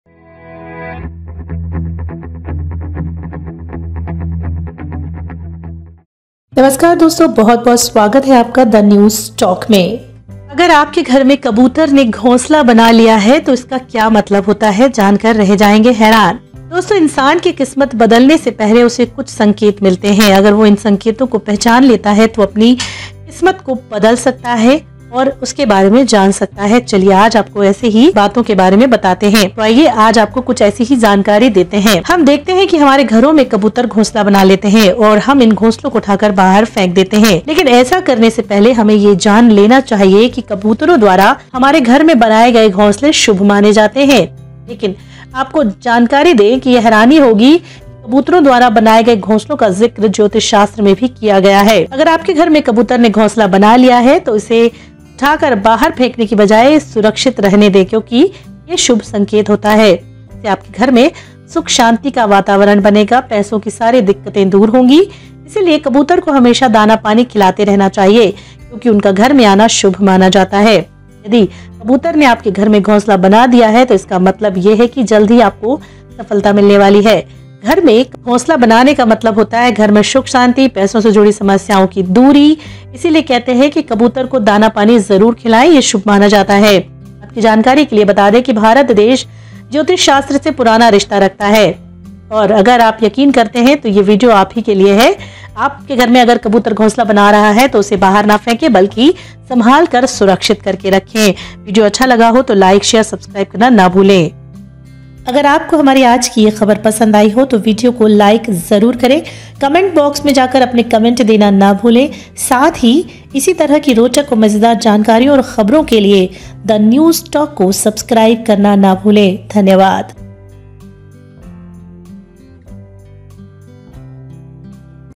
अगर आपके घर में कबूतर ने घोसला बना दिया है तो आपके साथ भी होगा ऐसा, जरूर देखे
Before changing the fate of human beings, there are some indications that if he recognizes these signals then he can know his changing fate in advance, so come on, let's meet friends today, we will tell you about some such signs.

We see that pigeons make nests in our homes and we throw them out and throw them out. But before we do this we should know that the ghosts made in our house by pigeons are very auspicious.

 But you will be surprised to know that the nests made by pigeons have also been mentioned in astrology.

If the pigeon has made the announcement in your house, then instead of throwing it out, let it be safe because it is auspicious for you. This will create an atmosphere of peace and happiness in your home. All problems of money will be solved.

 Therefore, pigeons should always keep feeding grains because they are considered to be auspicious in their home.

If a pigeon has made a nest in your house then it means that you will soon get success. Making a dove house nest in the house means that there will be peace in your house and all the problems related to money will be removed.

 Therefore the pigeon should always feed grain and water because it is considered very auspicious.